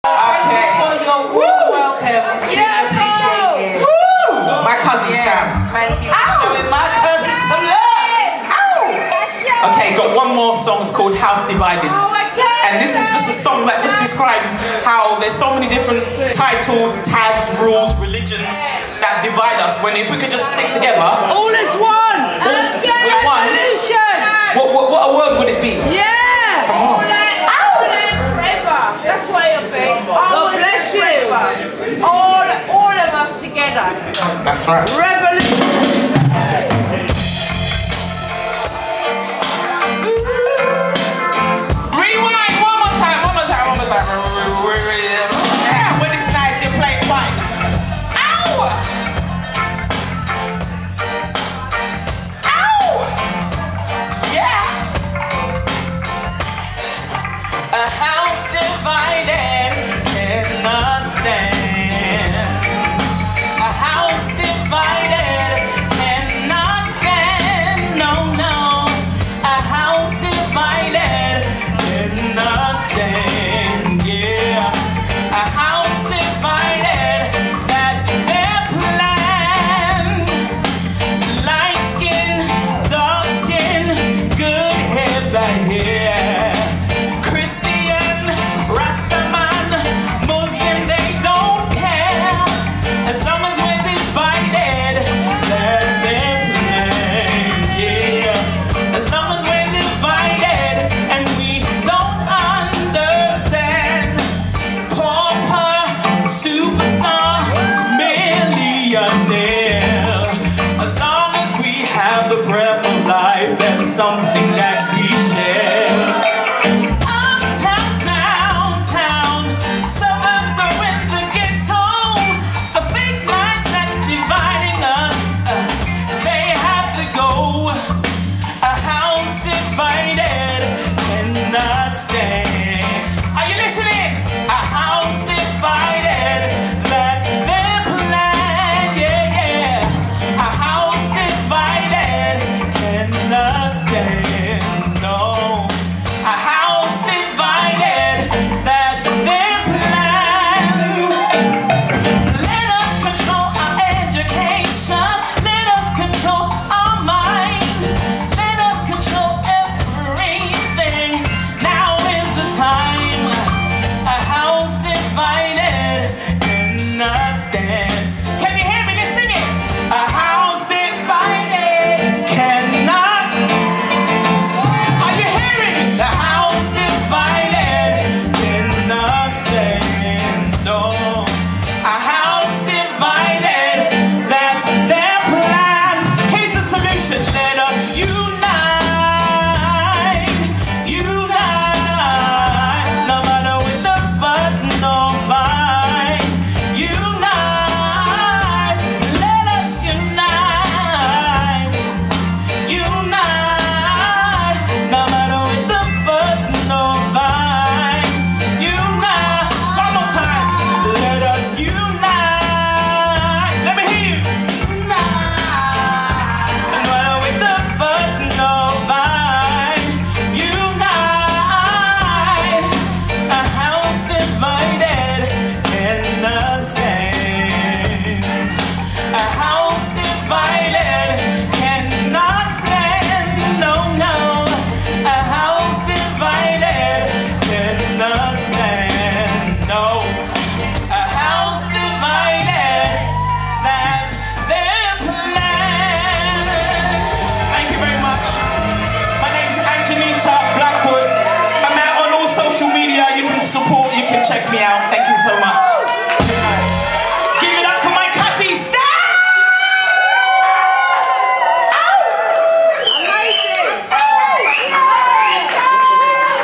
Empress Lyan S and Friends @ Hootananny 30Sep21 Pt 2 (Angenitta Blackwood)
Cecil Reuben events  Empress Lyan S & Friends  at the Hootananny, Brixton, London. Backed by the Natural Instinct band.   30th September, 2021. Hosted by Cecil Reuben & Lyan S. Part 2. Angenitta Blackwood.